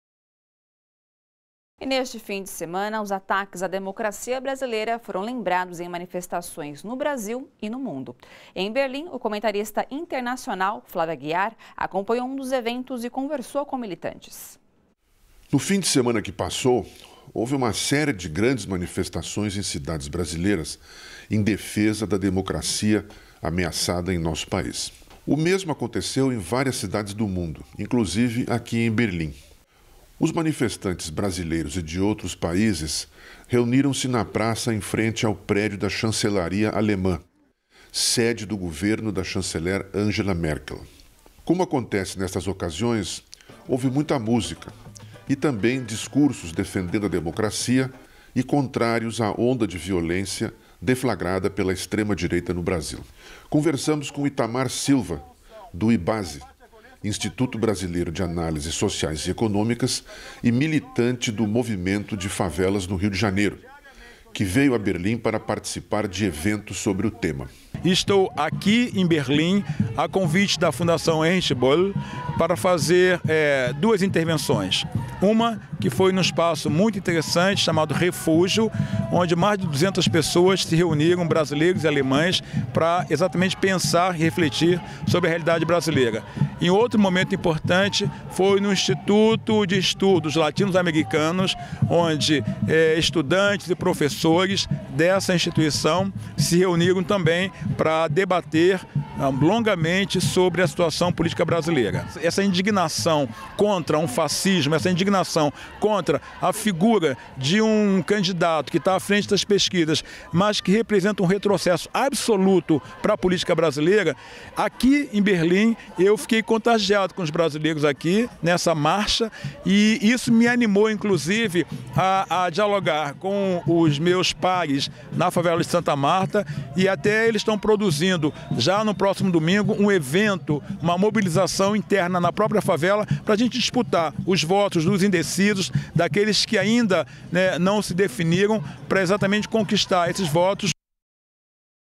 Mundo repercute ataques à democracia brasileira com atos contra o fascismo
No último fim de semana, os ataques à democracia brasileira foram lembrados em diversas manifestações no Brasil e no mundo. Em Berlim, o comentarista internacional Flávio Aguiar acompanhou um dos eventos e conversou com militantes.